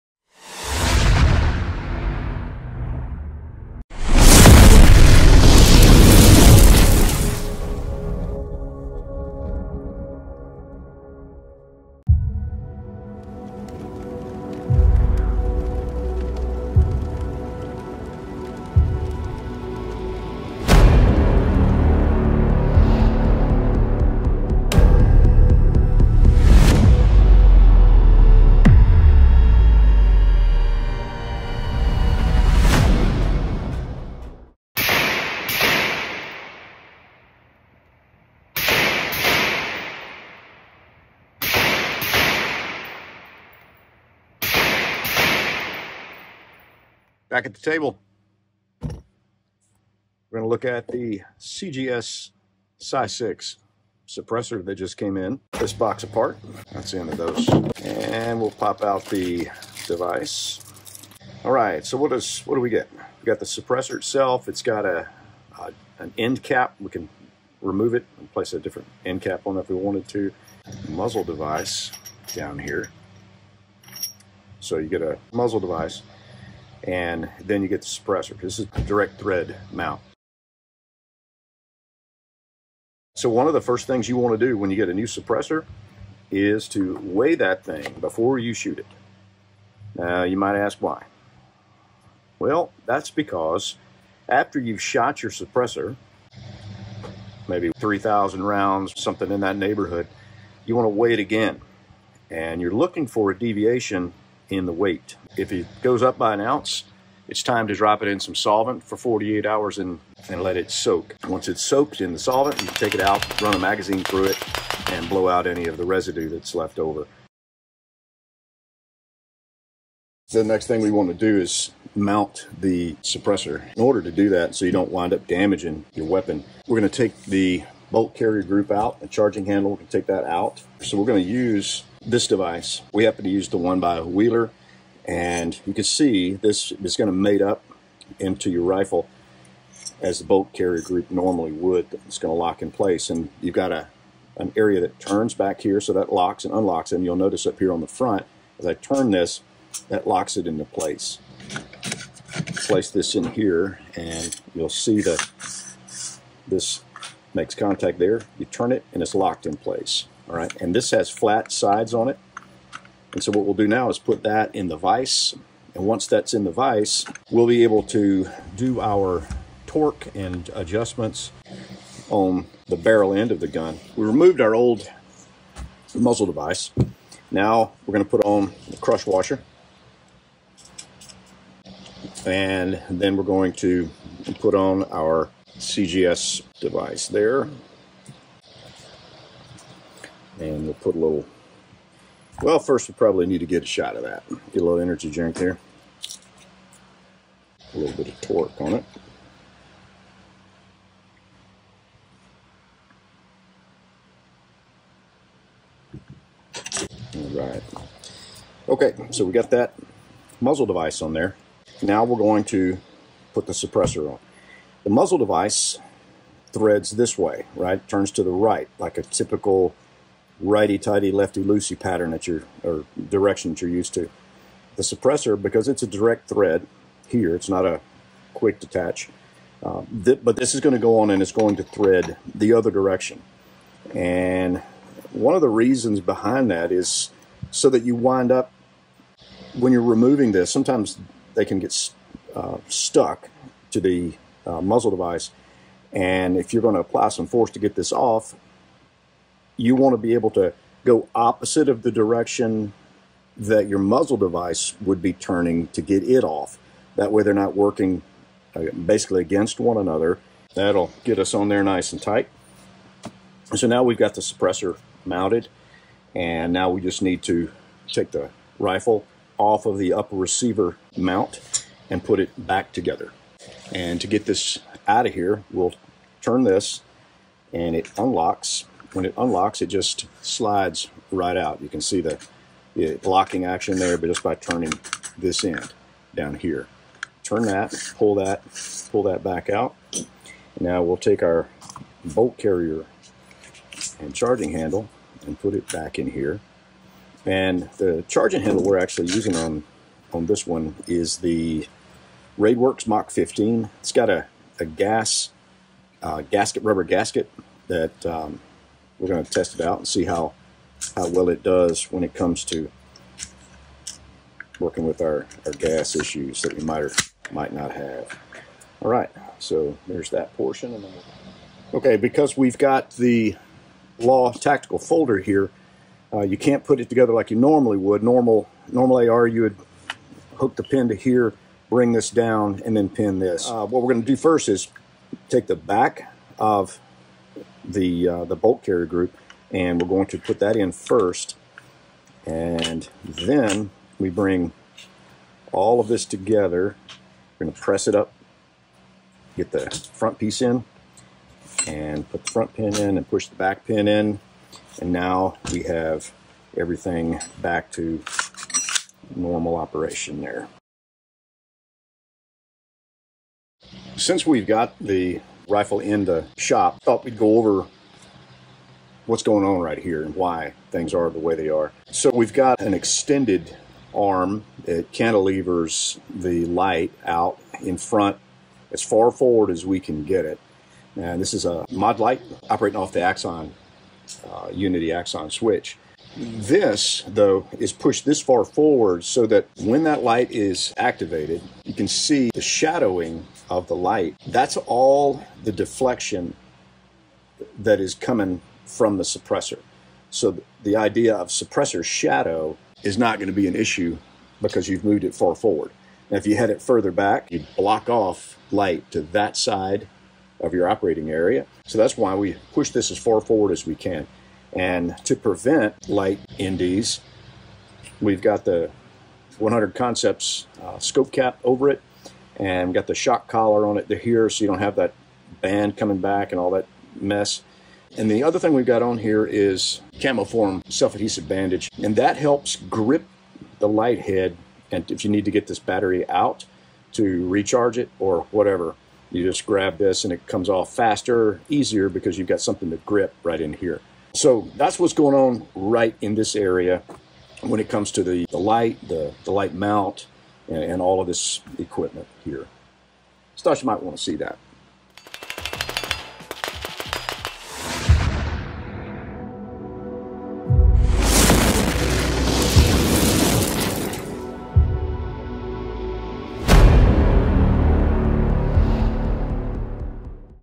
Suppressor Used By CIA Ground Branch Operator
Ever wonder what a CIA paramilitary / GRS operator uses to suppress his rifle? In the video games they use one, but in real life what's the choice?

In this video, a former operator goes into exactly what he uses and how to install it. We'll look at the CGS SCI-SIX dedicated 5.56mm suppressor.

AX YouTube: @am-kx
AX on Instagram: @amerkinetix

TIMELINE:

00:00 Intro
00:35 Shooting it
00:46 At the table
01:21 First thing to do
01:57 Installation
02:56 The muzzle device
03:30 The suppressor
04:57 The charging handle (Raidworx Mach 15)
06:01 Why things are set up the way they are...

*ADMIN NOTE: Crush washers don't crush uniformly, which could create an alignment problem, which has the outside potential for a baffle strike on a suppressor. Instead, apply Rocksett or a similar thread locker to the barrel threads. The SCI-SIX has a rather generous bullet path (you can actually shoot larger calibers than 556 thru it, but we didn't say that and don't suggest trying it). So to be on the safest side, ditch the crush washer if you want.